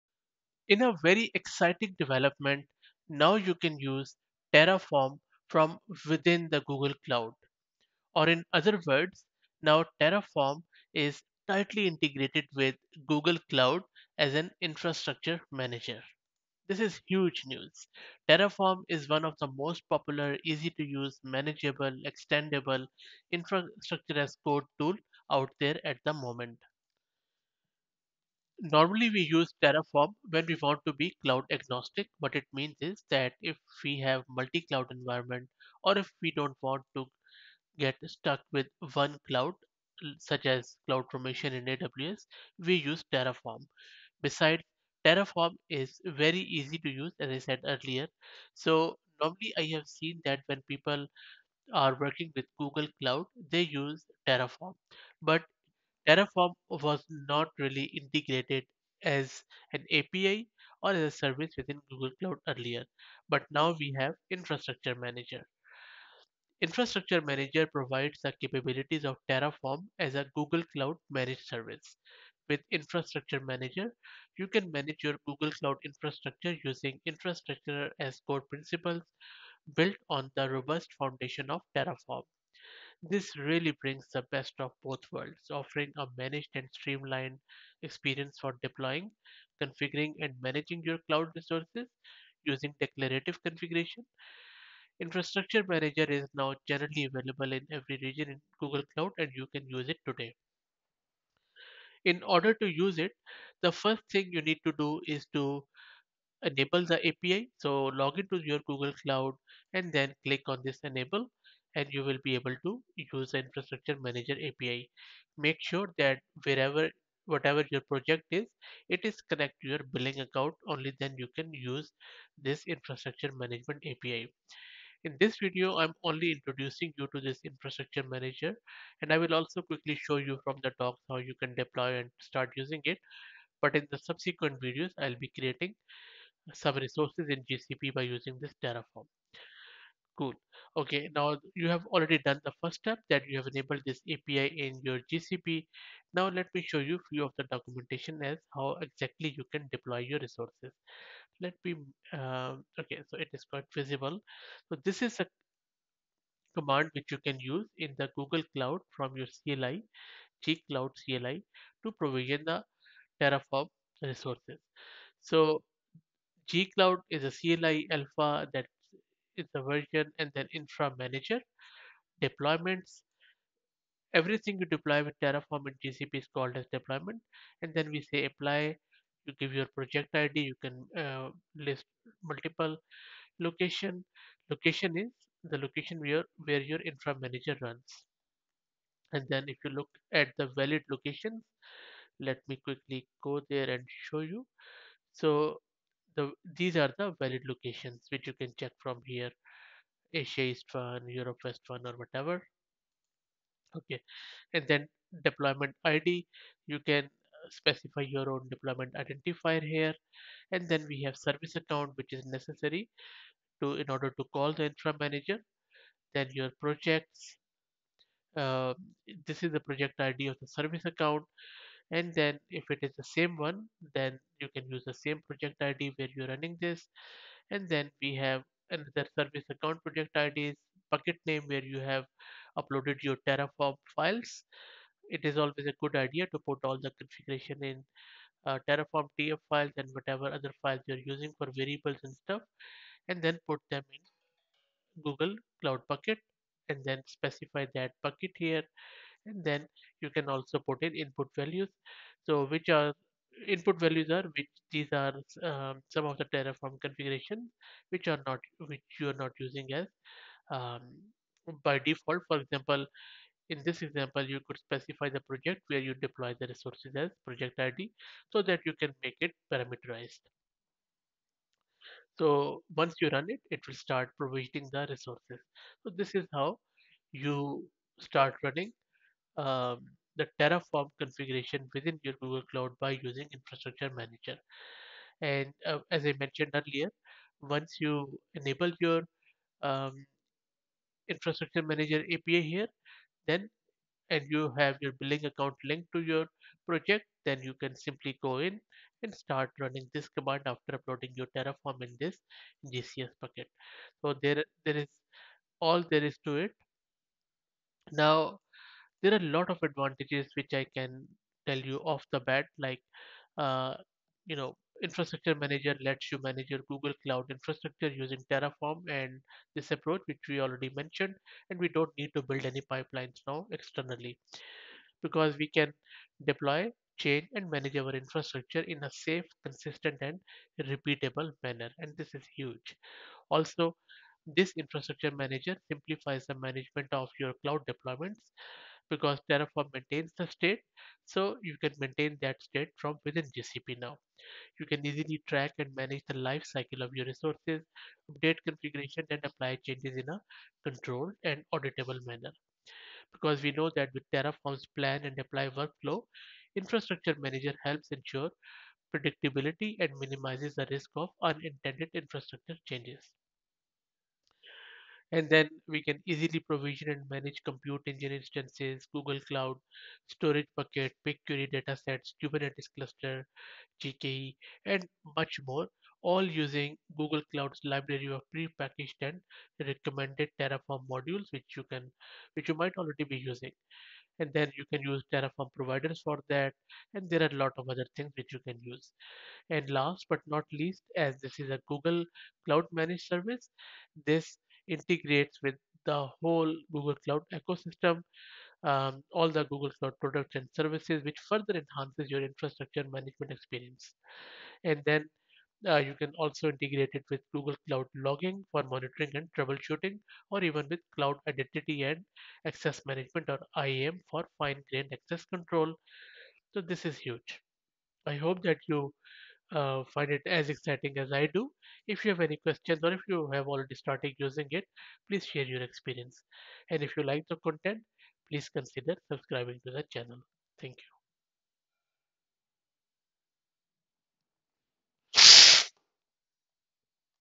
Terraform Infrastructure Manager in Google Cloud
This video introduces Infrastructure Manager in GCP, which provides the capabilities of Terraform as a Google Cloud managed service. With Infrastructure Manager, you can manage your Google Cloud infrastructure using Infrastructure as Code (IaC) principles, built on the robust foundation of Terraform.